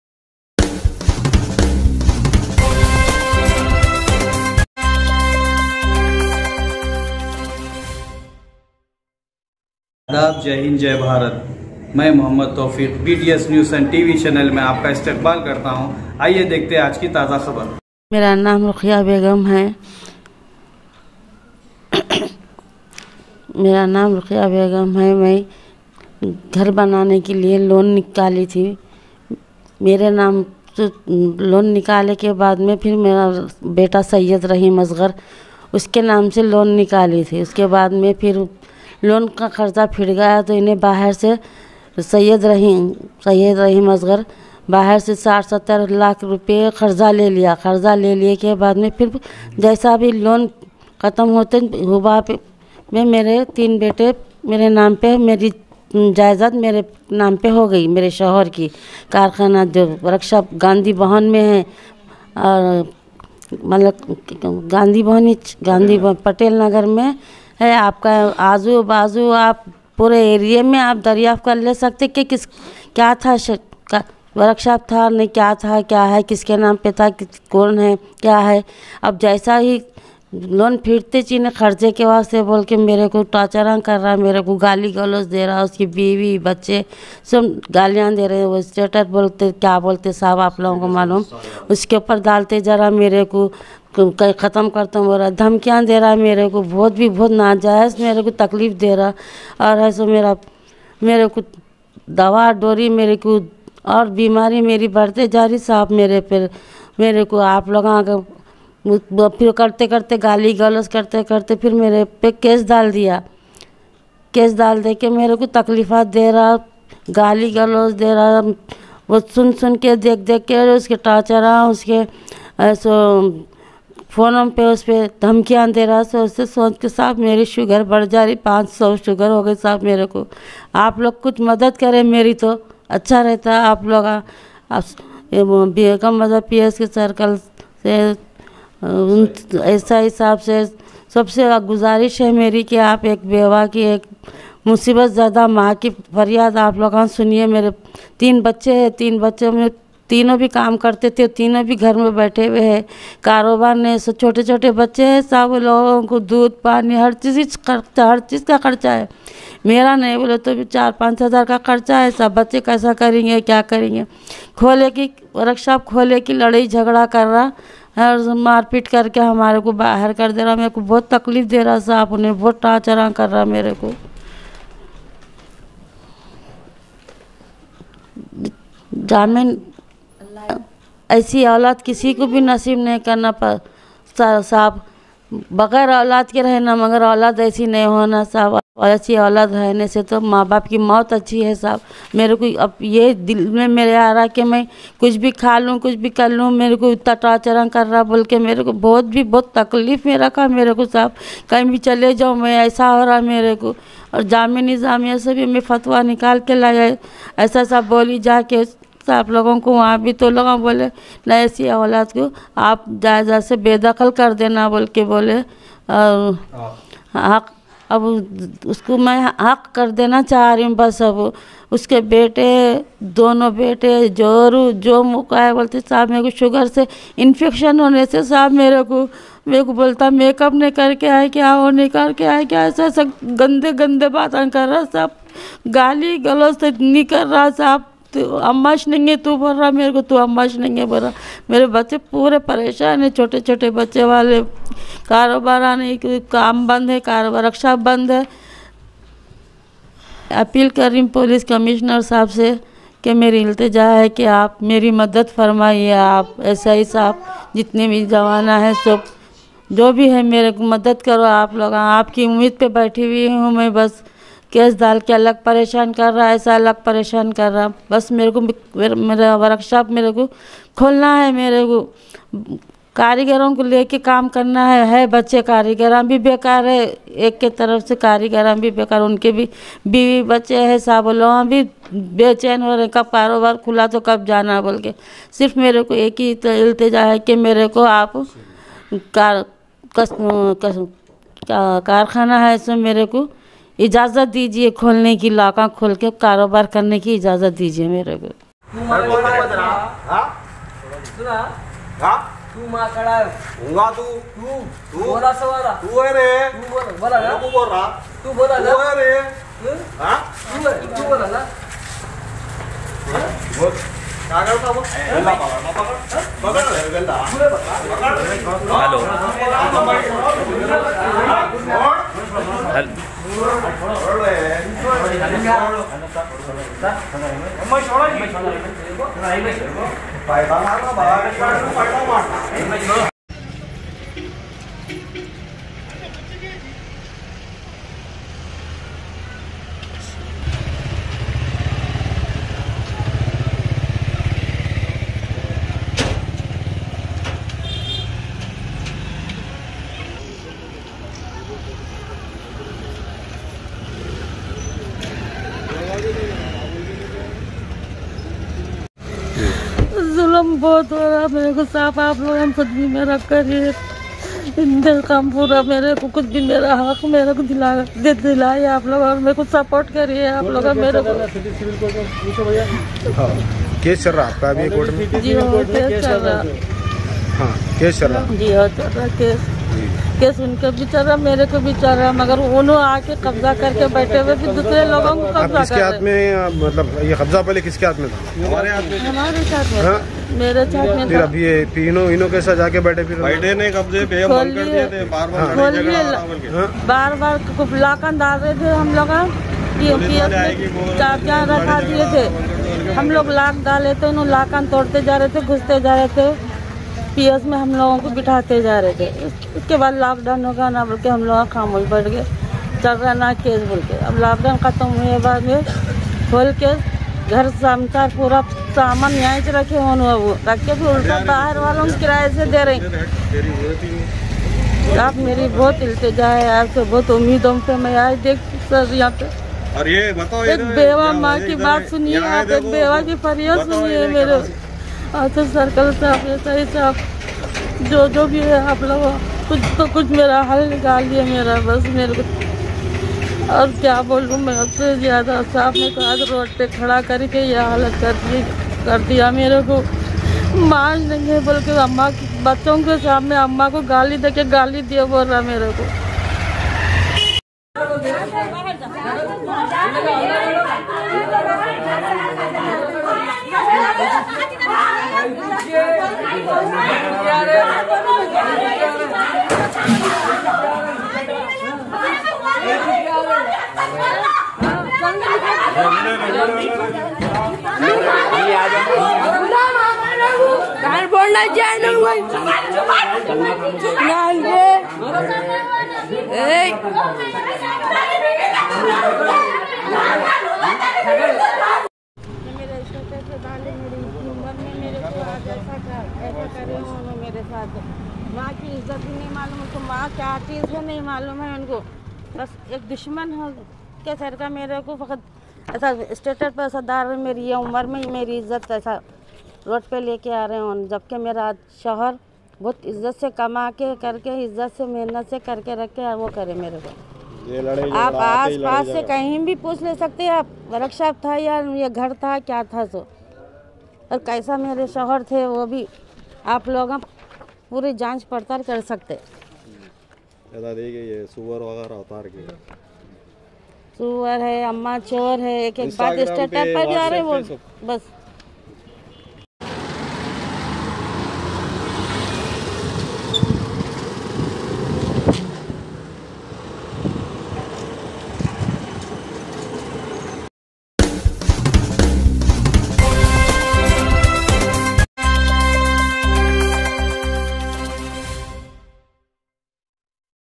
Allah Aisi Aulad Kisi Ko Na Dein Ek Maa Ki Ilteja..i Maa Ko Maarne Ki Di Dhamki..Daahoufa
Allah Aisi Aulad Kisi Ko Na Dein Ek Maa Ki Ilteja..

Property Ko Lekar Bete Ne Apni Maa Ko Maarne Ki Di Dhamki..

Dargah Yousufain Nampally Kay Kareeb  Rehne Ne Wali 60 Sala Ruqiya Begum Ko 4 Bete Aur 1 Beti Hai.

Ruqiya Begum Kay Shohar Ka Inteqal 2015 Mein Huwa Uskay Baad Saari Property Ruqiya Begum Kay Nam Per Hogai..

Uskay Baad Ruqiya Begum Ne Ghar Banane Kay Liye Gandhi Bhavan Patel Nagar Mein Jo  General Motors Car Work Shop Hai Usper Bade Bete Syed Raheem Asghar Kay Nam Per Partition Deed Banakar Loan Liya Tha..

Loan Qatam Hone Kay Baad Hiba Nama Kar Kay Wapis Wo Workshop Ki Property Ruqiya Begum Kay Nam Per Hogai..

Ruqiya Begum  Ka Kehna Hai Kay Uskay Baad Unkay Bade Bete Syed Raheem Asghar Ki Niyat Kharab Hogai Aur Usne Poori Property Ko Hadap Ne Kay Liye City Civil Court Mein Case  Daldiya Aur Mujhe Pareshan Kar Raha Hai Aur Maarne Ki Bhi Dhamki Deraha Hai ..

Ruqiya Begum Ne Commissioner Of Hyd Sri . CV Anand IPS  Se Appeal Ki Hai Kay Wo Inki Madad Kare ..

Suniye Saari Tafseel Unhi Ki Zubani..